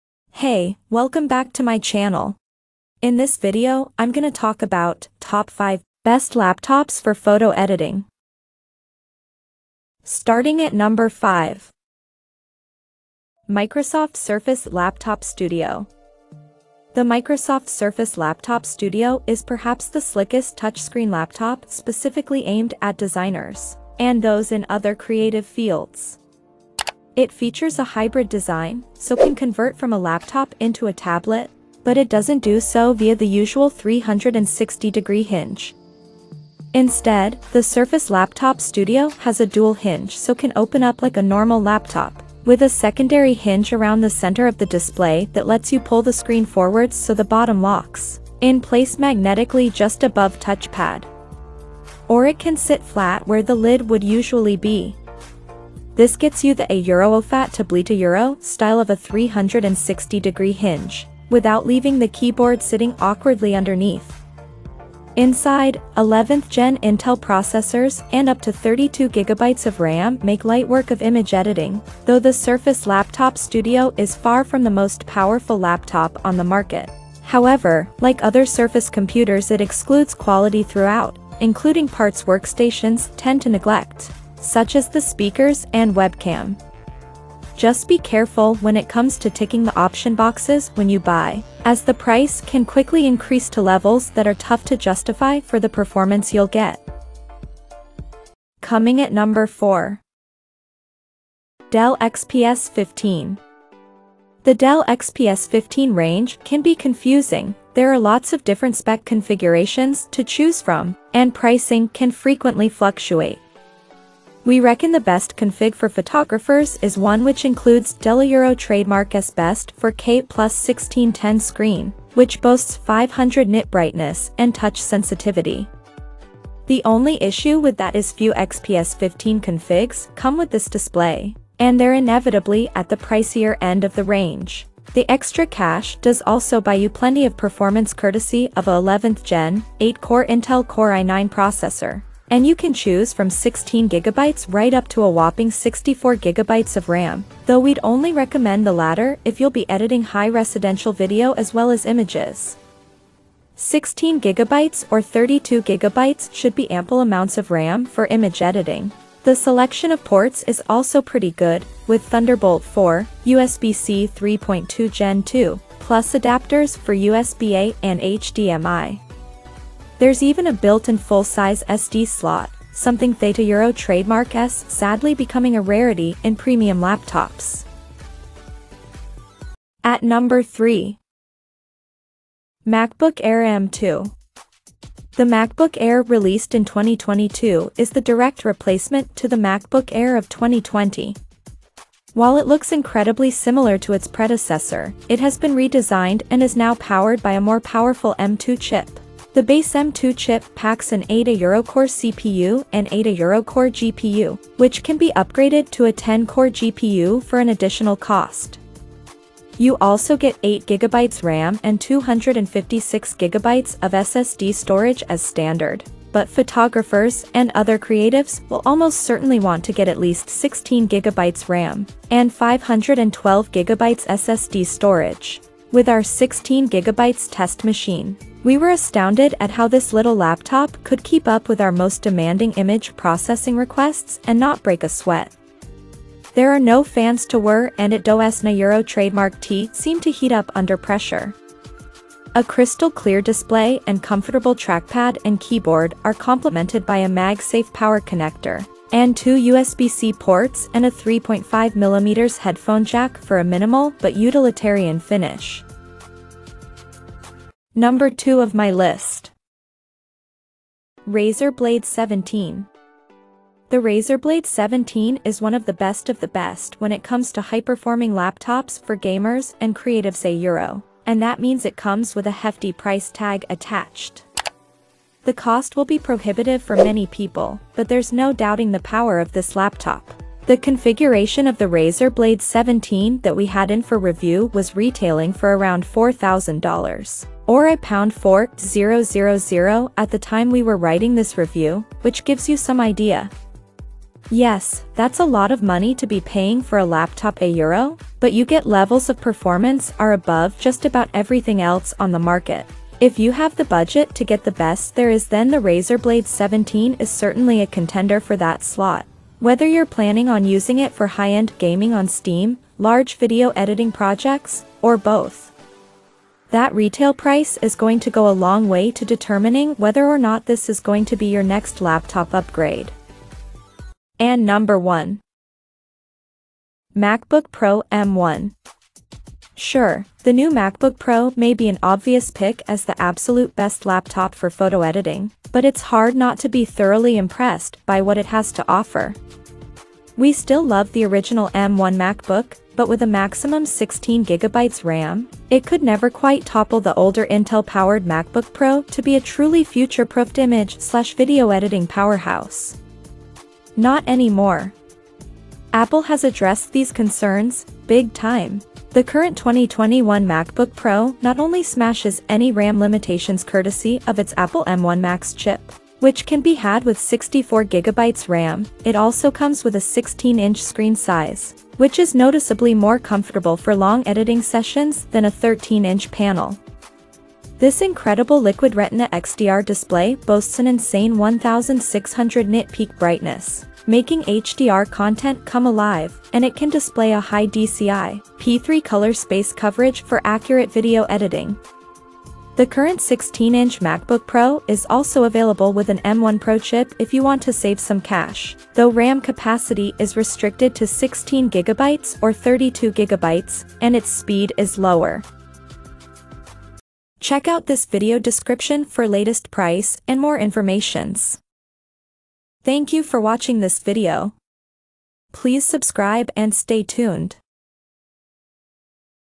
5 Best Laptops For Photo Editing in 2023
"Here is the Updated Prices of the Best Laptops For Photo Editing in 2023!

Things that we mentioned in this video:
1. MacBook Pro M1 - B09JQKBQSB
2. Razer Blade 17 - B08SJJ8H7Z
3. MacBook Air M2 - B0B3BVWJ6Y
4. Dell XPS 15 - B089HJX59L
5. Microsoft Surface Laptop Studio - B07XC2XGR9"